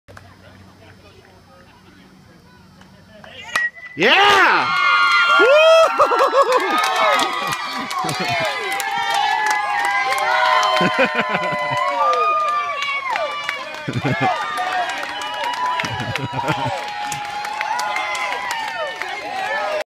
Jake Bell Smashes a Double
My son Jake kicking butt and having a blast!